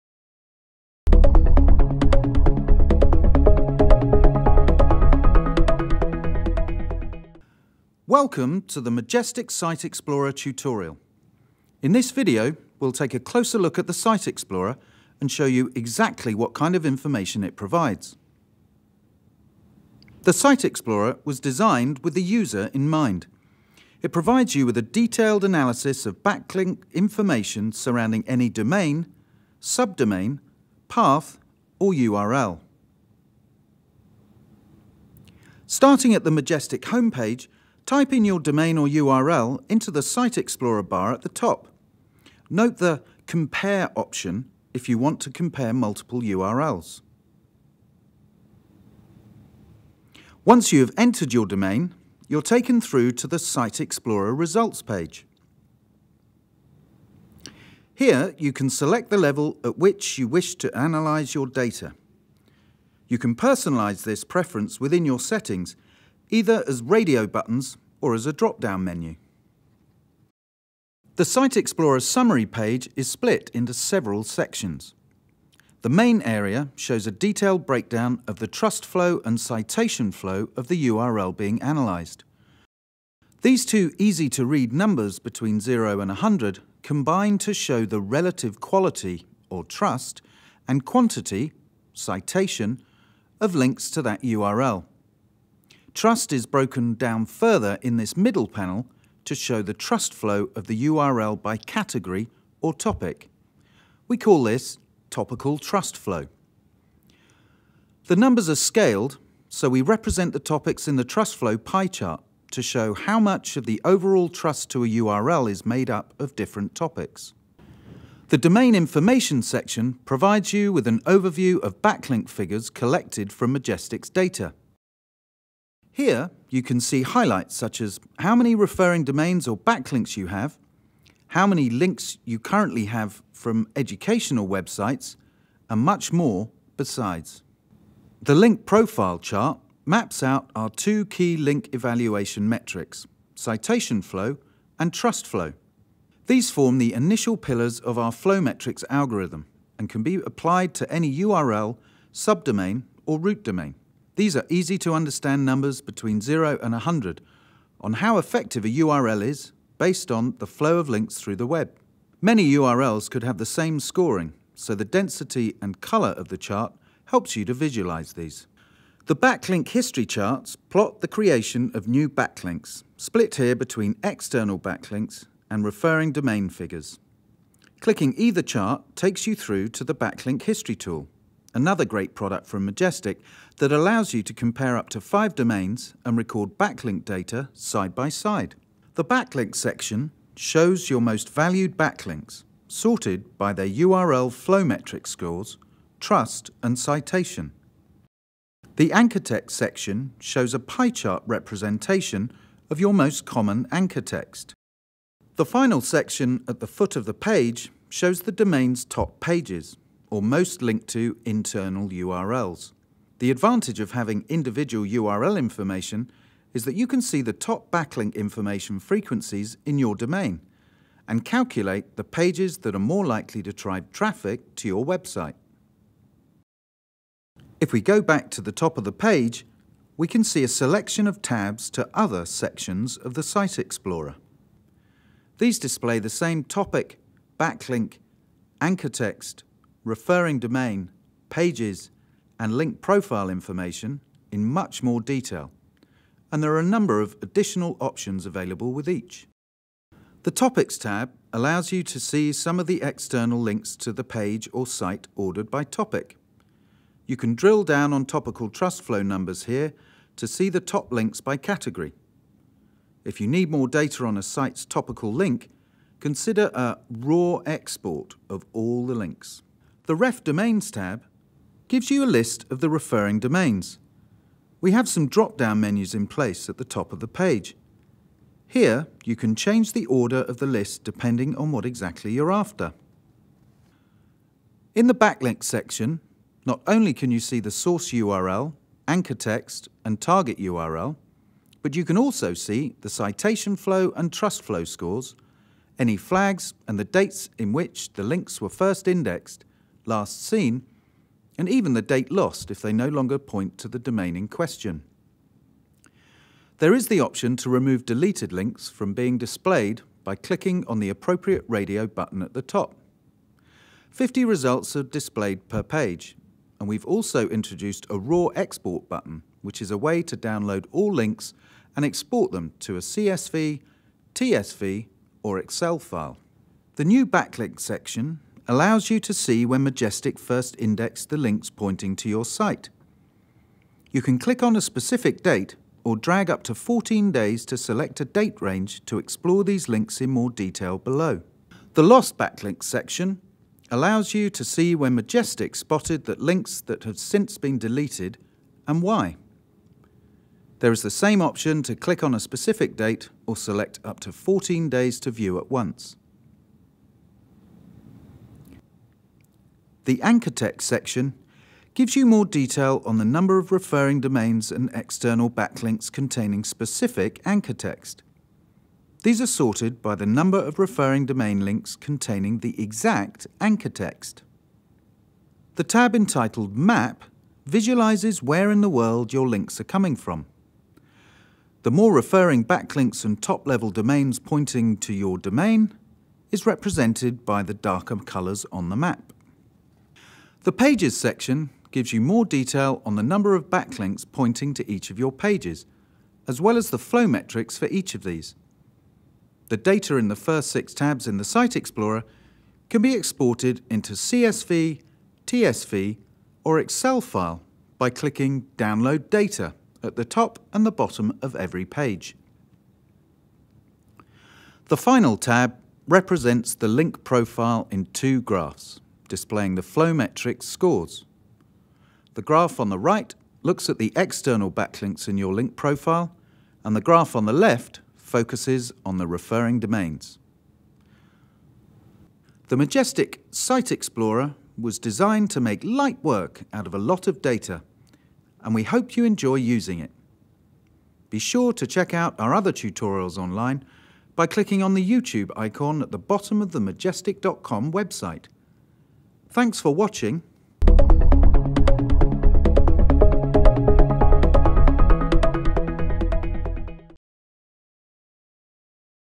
Majestic Tutorial: How to Use the Site Explorer (2015)
In this 5-minute walkthrough video you will learn what Majestic’s Site Explorer - one of Majestic’s main tools – does and how you can analyse your website and its backlink profile information.
for more tips and tricks!

The Site Explorer was designed with the user in mind. It provides you with a detailed analysis of backlink information surrounding any domain, subdomain, path or URL.
Starting at the Majestic home page, type in your domain or URL into the site explorer bar at the top. Note the “Compare” option if you want to compare multiple URLs. Once you have entered your domain, you are taken through to the Site Explorer.  Here you can select the level at which you wish to analyse your data and tons of useful tools for you to explore the link data of the website you are analyzing.